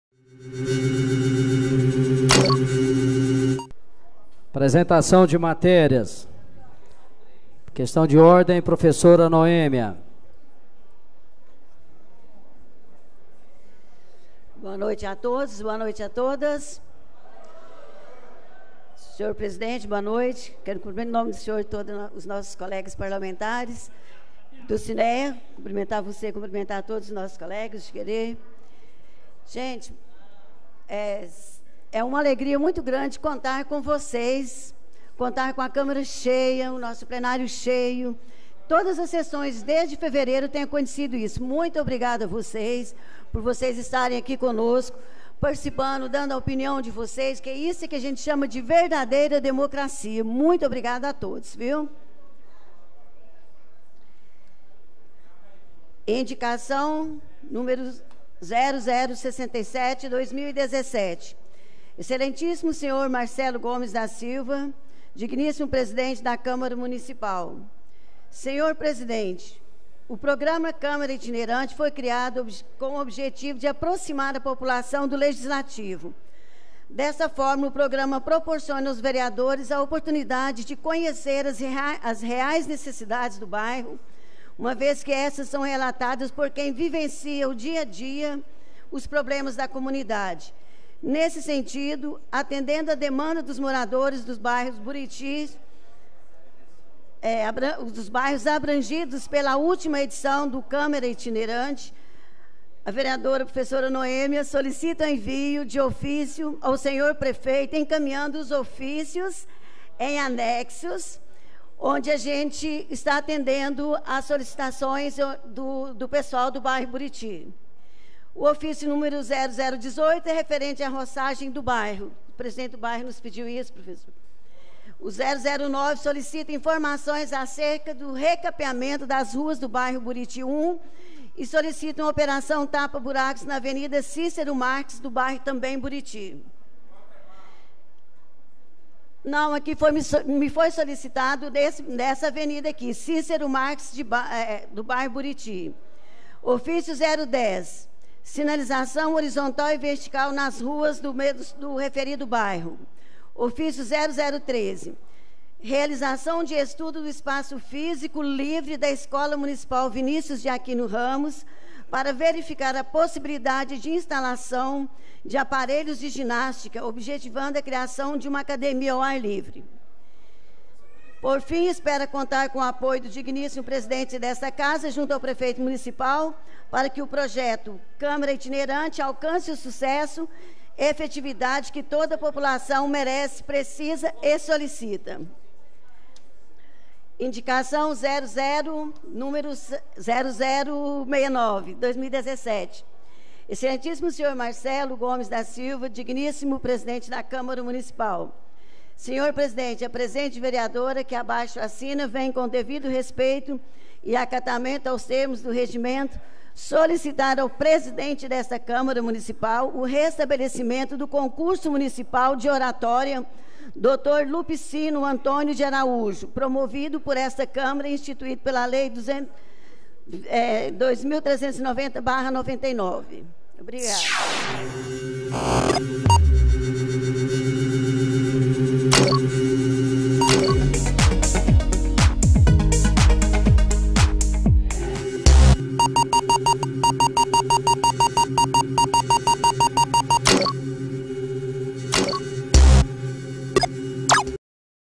SESSÃO 11 (03ABR2017)-05-PROF NOÊMIA-APRESENTA MATÉRIAS
Sequência Apresentação de Matérias - Vereadora Professora Noêmia  apresenta as Indicações 67 e 69, durante a Sessão Ordinária 11 realizada no plenário da Câmara Municipal de Itumbiara-GO, na noite de 03 de abril de 2017.